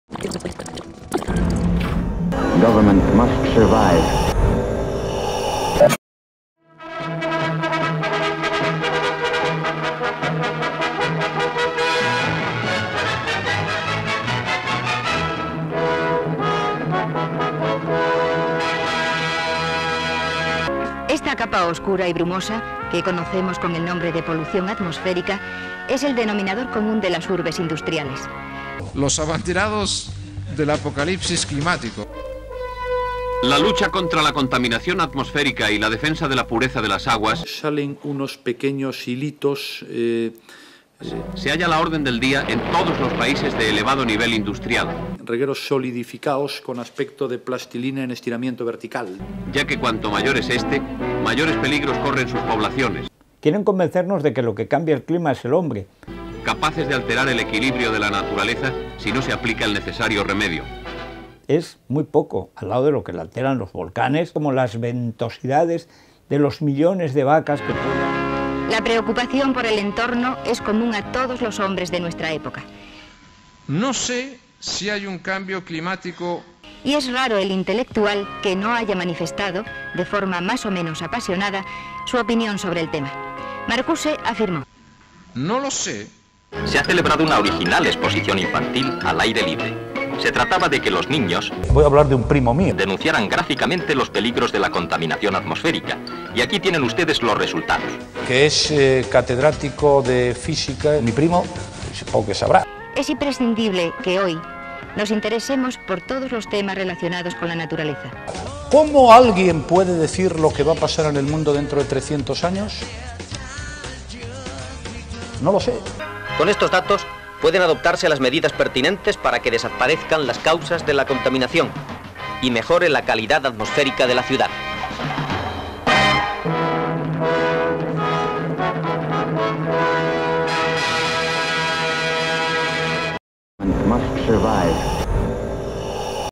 Regueros Solidificados Con Aspecto De Plastilina En Estiramiento Vertical
Quieren convencernos de que lo que cambia el clima es el hombre... [capaces de alterar el equilibrio de la naturaleza si no se aplica el necesario remedio] es muy poco al lado de lo que la alteran los volcanes, como las ventosidades de los millones de vacas. [la preocupación por el entorno es común a todos los hombres de nuestra época] No sé si hay un cambio climático.